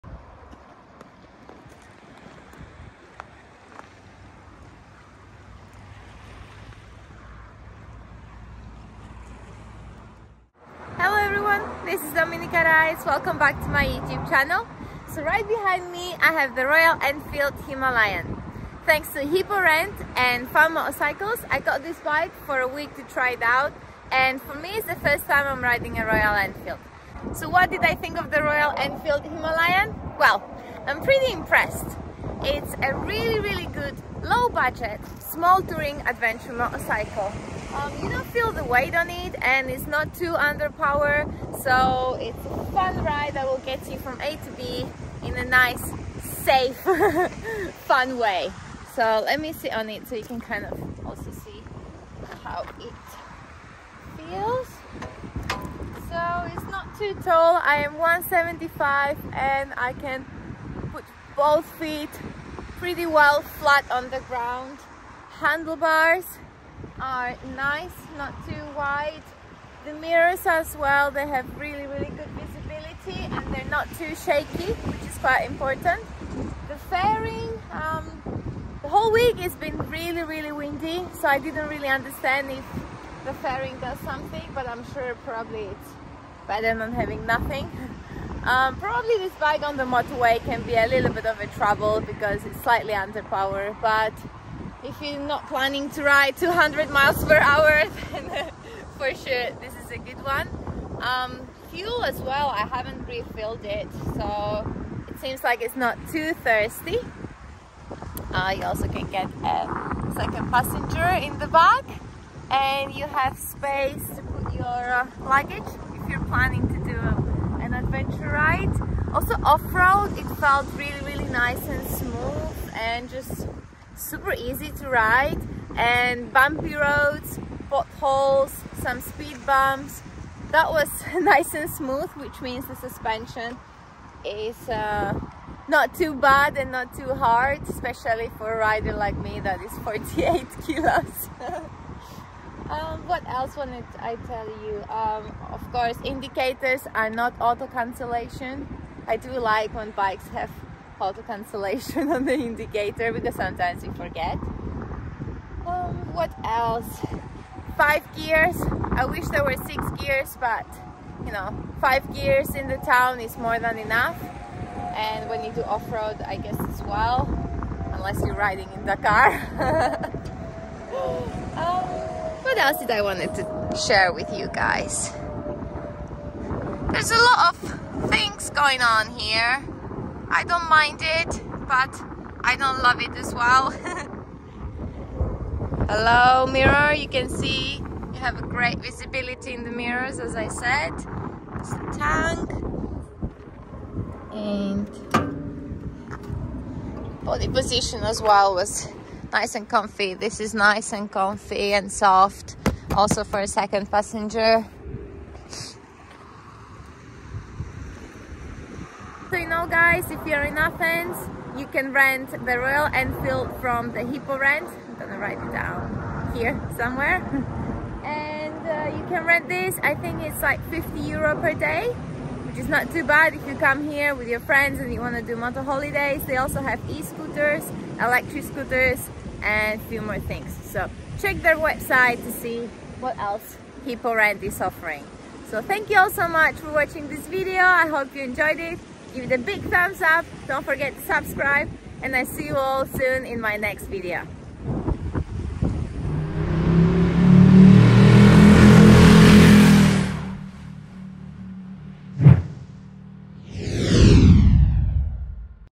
My thoughts after riding the Royal Enfield Himalayan rental bike from Hippo Rent/Dominika Rides
Hello everyone!
I’m back with another video, this time
I test rode for few days the Royal Enfield Himalayan bike from Hippo Rent in Athens & here you have my thoughts about it!

See y’all in my next one!!!